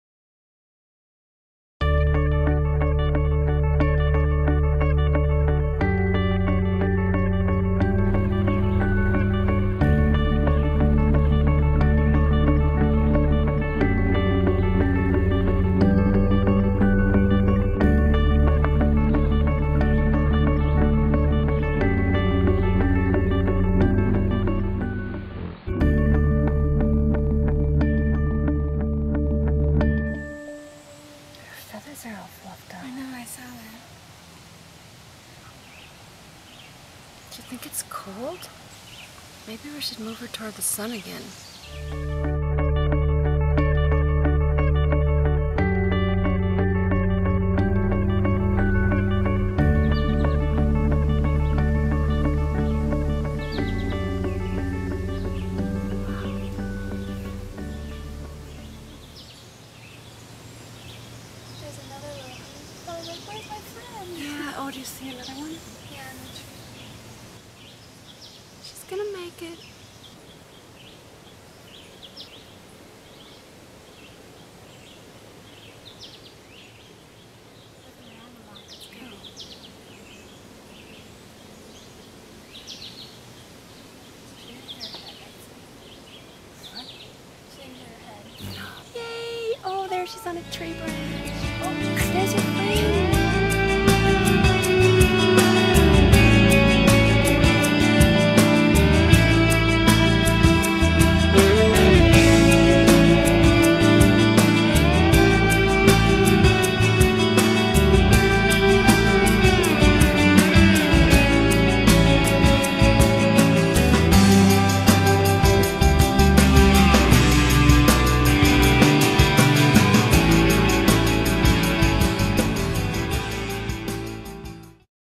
Hummingbird Was Stunned After Flying into a Window
I found this hummingbird on my deck with her wings spread straight out and she was not moving, I thought she was dead...probably flew into the window. I picked her up and a neighbor and I  watched her for about 20 min. till she finally fly away, We were happy to see her land on a tree branch where another hummingbird was waiting for her. She sat there for about another 5 min. and then flew away. I'd like to think that I see her at my hummingbird feeder now and then.